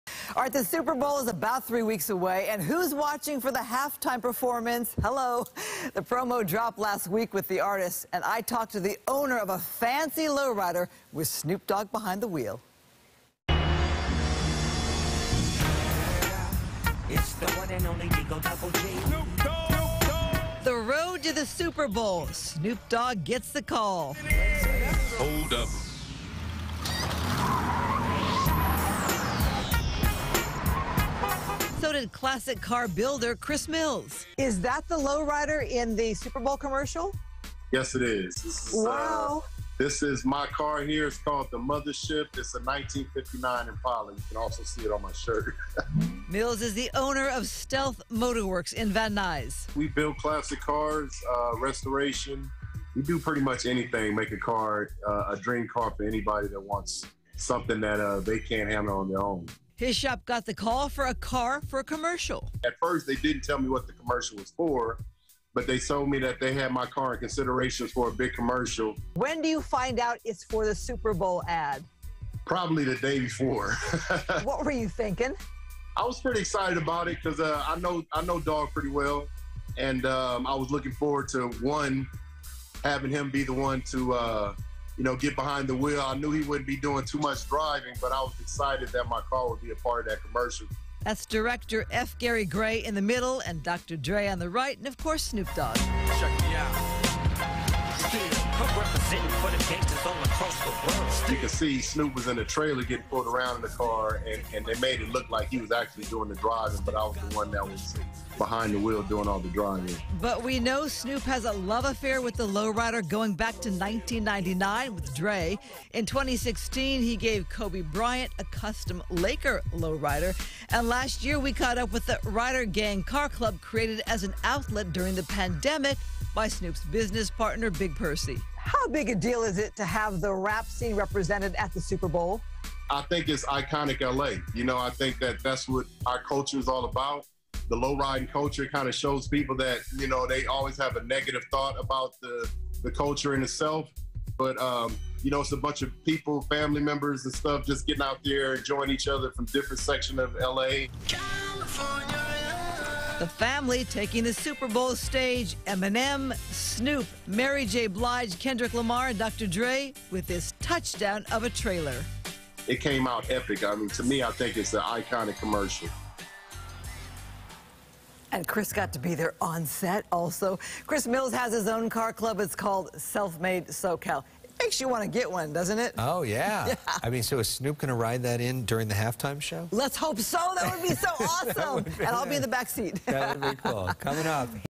Snoop Dogg drives lowrider from Van Nuys shop for Super Bowl LVI halftime commercial
FOX 11 spoke with the Van Nuys-based custom car builder as all eyes will be on his lowrider during one of the Super Bowl halftime commercials.